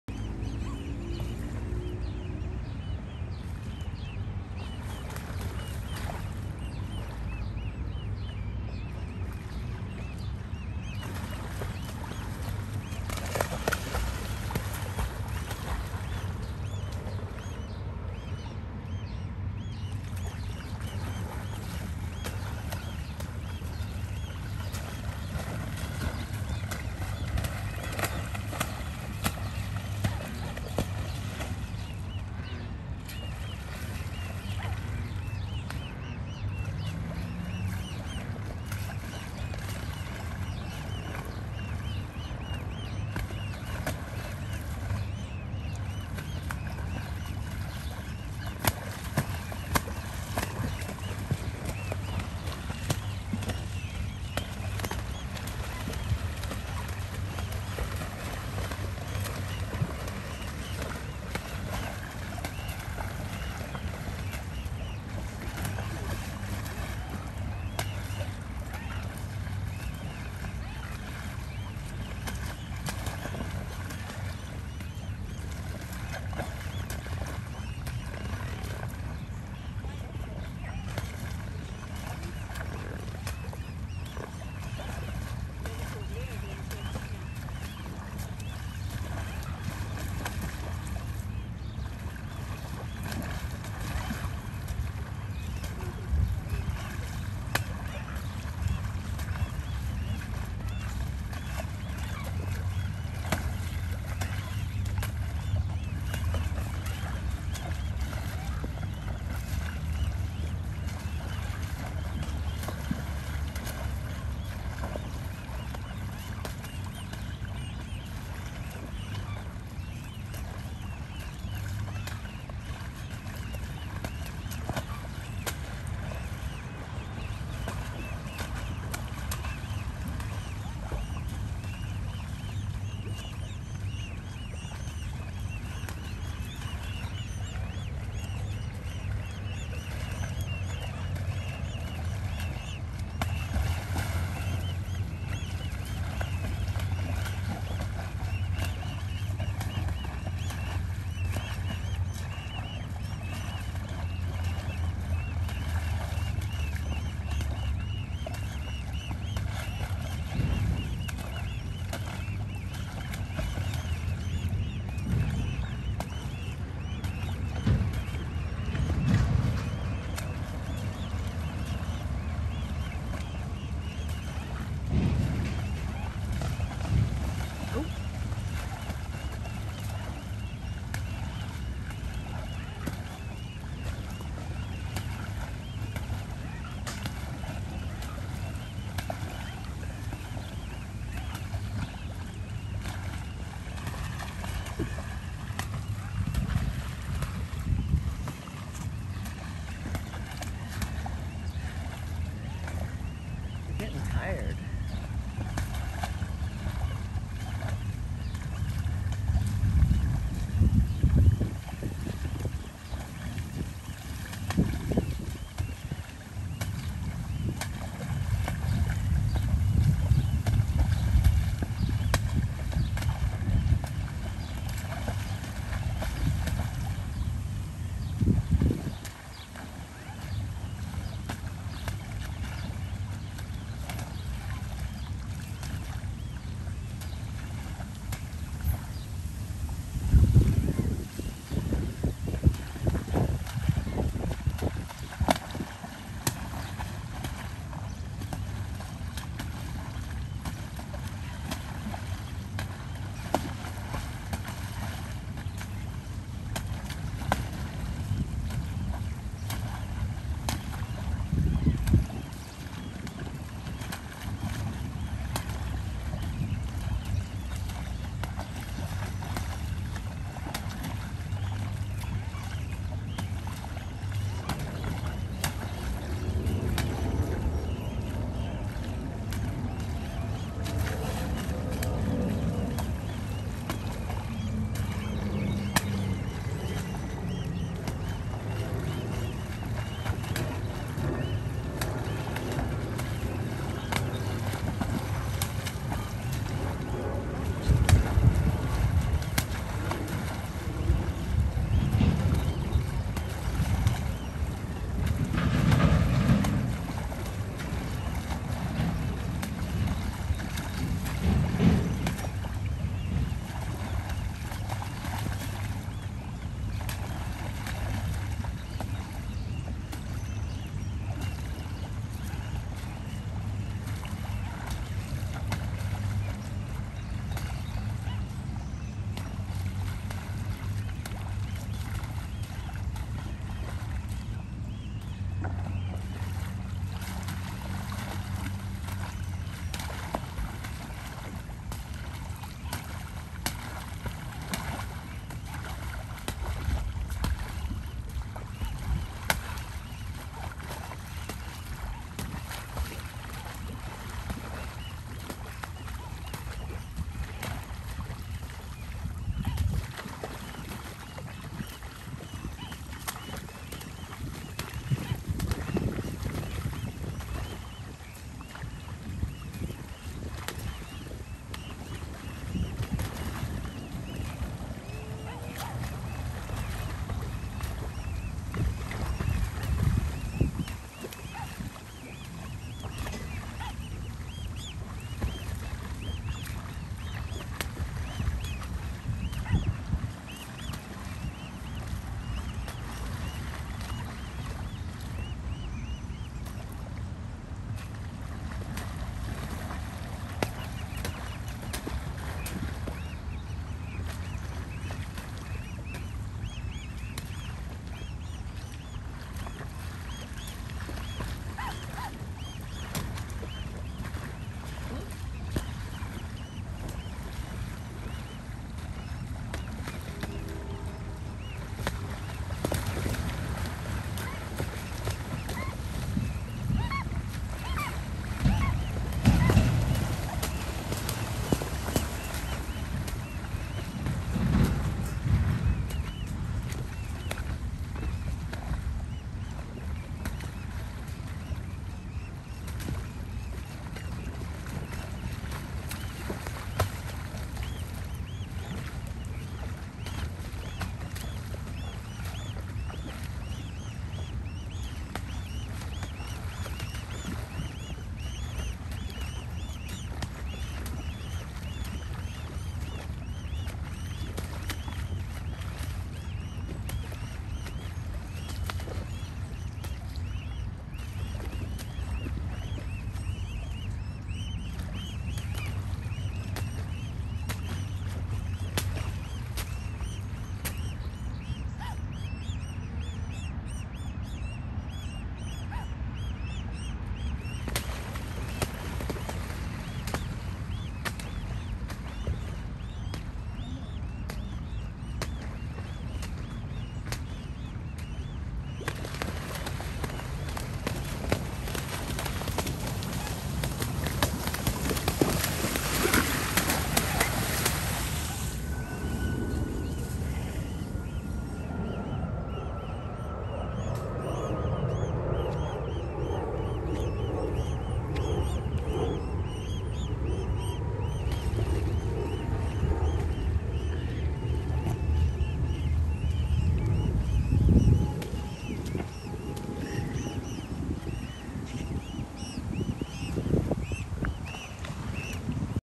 Swan Battle part 2
On my walk this morning I encountered this epic swan battle!